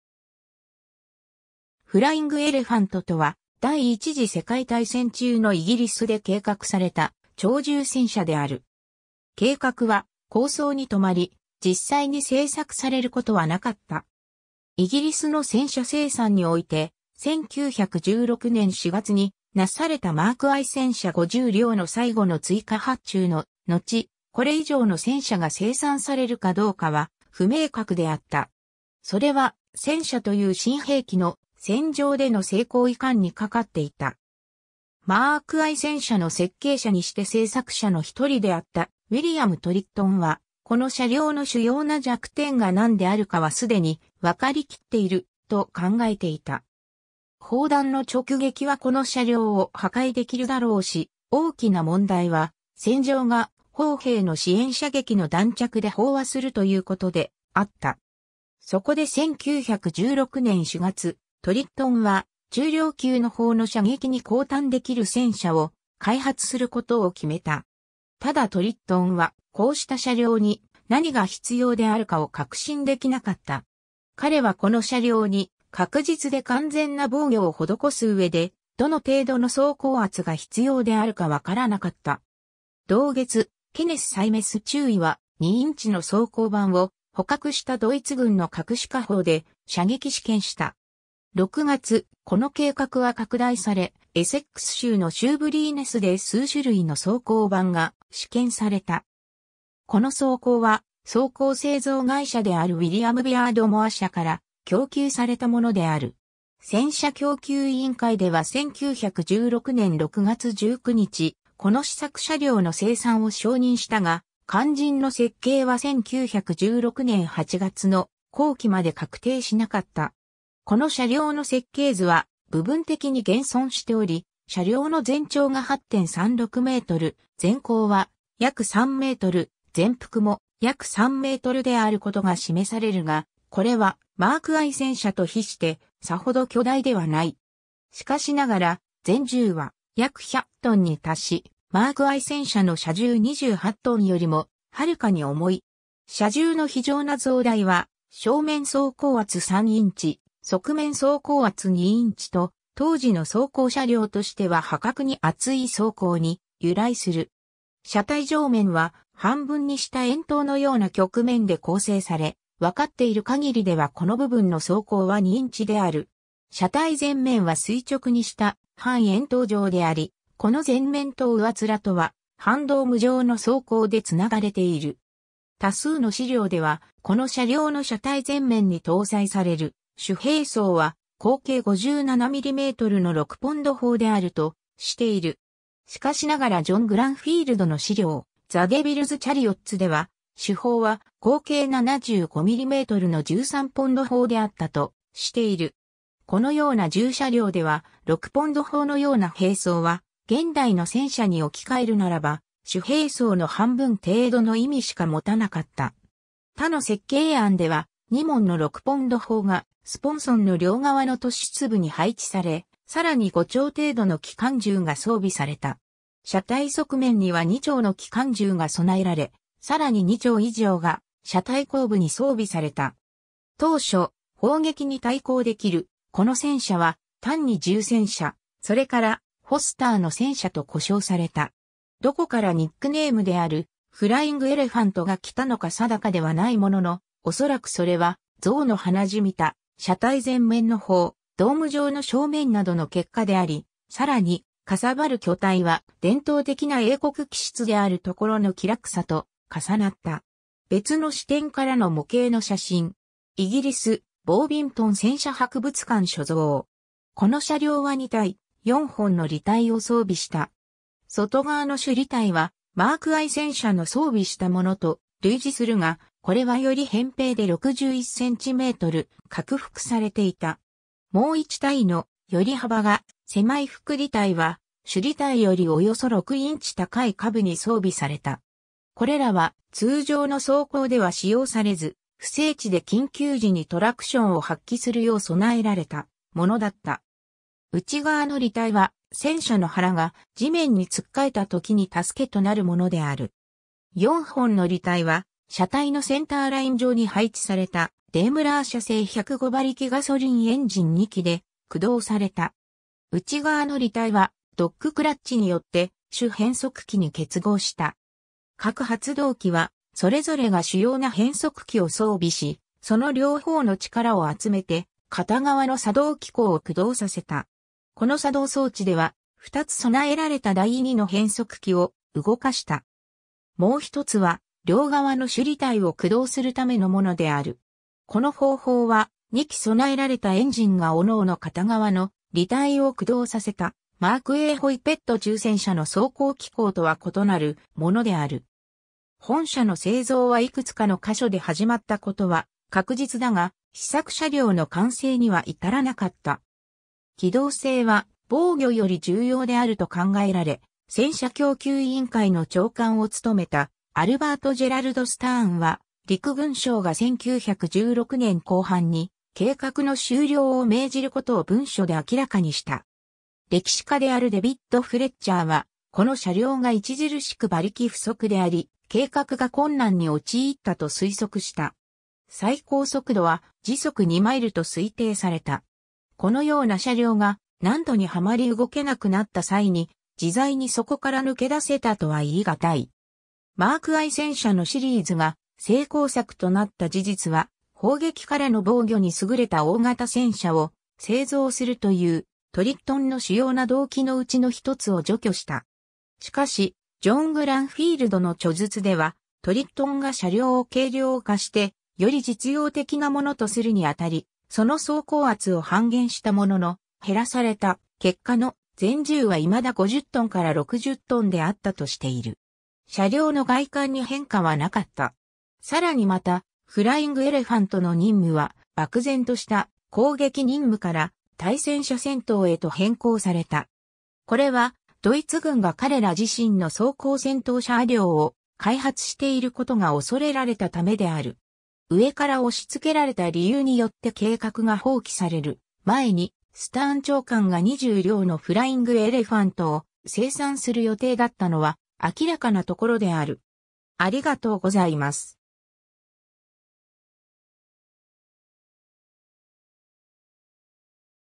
フライング・エレファント
フライング・エレファント（英語: Flying Elephant）とは、第一次世界大戦中のイギリスで計画された超重戦車である。
計画は構想に留まり、実際に製作されることはなかった。
 イギリスの戦車生産において、1916年4月になされたマーク I 戦車50輌の最後の追加発注ののち、これ以上の戦車が生産されるかどうかは不明確であった。
それは、戦車という新兵器の、戦場での成功如何にかかっていた。
マーク I 戦車の設計者にして製作者の一人であったウィリアム・トリットンは、この車輛の主要な弱点が何であるかは既にわかりきっている、と考えていた。
砲弾の直撃はこの車輛を破壊できるだろうし、大きな問題は、戦場が砲兵の支援射撃の弾着で飽和するということであった。
そこで1916年4月、トリットンは、中量級の砲の射撃に抗堪できる戦車を開発することを決めた。
 ただトリットンは、こうした車輛に何が必要であるかを確信できなかった。
彼はこの車輛に、確実で完全な防御を施すうえで、どの程度の装甲厚が必要であるか判らなかった。
同月、ケネス・サイメス中尉は、2インチ（51mm）の装甲板を、捕獲したドイツ軍の各種火砲で射撃試験した。
6月、この計画は拡大され、エセックス州のシューブリーネスで数種類の装甲板が試験された。
この装甲は装甲製造会社であるウィリアム・ビアードモア社から供給されたものである。
戦車供給委員会では1916年6月19日、この試作車両の生産を承認したが、肝心の設計は1916年8月の後期まで確定しなかった。
 この車輛の設計図は部分的に現存しており、車輛の全長が8.36メートル、全高は約3メートル、全幅も約3メートルであることが示されるが、これはマーク I 戦車と比してさほど巨大ではない。
しかしながら全重は約100トンに達し、マーク I 戦車の車重28トンよりもはるかに重い。
車重の非常な増大は、正面装甲厚3インチ（76.2mm）、側面装甲厚2インチ（51mm）と、当時の装甲車両としては破格に厚い装甲に由来する。
車体上面は、半分にした円筒のような曲面で構成され、わかっているかぎりではこの部分の装甲は2インチ（51mm）である。
車体前面は垂直にした半円筒状であり、この前面と上面とは半ドーム状の装甲でつながれている。
 多数の資料では、この車輛の車体前面に搭載される主兵装は、口径57mmの6ポンド砲であるとしている。
しかしながらジョン・グランフィールドの資料『The Devil's Chariots』では、主砲は口径75mmの13ポンド砲であったとしている。
このような重車輛では、6ポンド砲のような兵装は、現代の戦車に置き換えるならば主兵装の半分程度の意味しか持たなかった。
他の設計案では、2門の6ポンド砲が、スポンソンの両側の突出部に配置され、さらに5挺程度の機関銃が装備された。
車体側面には2挺の機関銃が備えられ、さらに2挺以上が車体後部に装備された。
当初、砲撃に対抗できるこの戦車は、単に重戦車、それからフォスターの戦車と呼称された。
どこからニックネームである「フライング・エレファント」が来たのか定かではないものの、おそらくそれは象の鼻じみた車体前面の砲、ドーム状の正面などの結果であり、さらに、かさばる巨体は伝統的な英国気質であるところの気楽さと重なった。
 別の視点からの模型の写真。
イギリス、ボービントン戦車博物館所蔵。
 この車輛は2対、4本の履帯を装備した。
外側の主履帯はマーク I 戦車の装備したものと類似するが、これはより扁平で61センチメートル拡幅されていた。
もう1対の、より幅が狭い副履帯は、主履帯よりおよそ6インチ（約15cm）高い下部に装備された。
これらは通常の走行では使用されず、不整地で緊急時にトラクションを発揮するよう備えられたものだった。
内側の履帯は、戦車の腹が地面につっかえたときに助けとなるものである。
4本の履帯は、車体のセンターライン上に配置された、デイムラー社製105馬力（78キロワット）ガソリンエンジン2基で駆動された。
内側の履帯はドッグクラッチによって主変速機に結合した。
各発動機はそれぞれが主要な変速機を装備し、その両方の力を集めて片側の差動機構を駆動させた。
この差動装置では、2つ備えられた第二の変速機を動かした。
もう1つは両側の主履帯を駆動するためのものである。
この方法は、2基備えられたエンジンがおのおの片側の履帯を駆動させた、マーク A ホイペット中戦車の操向機構とは異...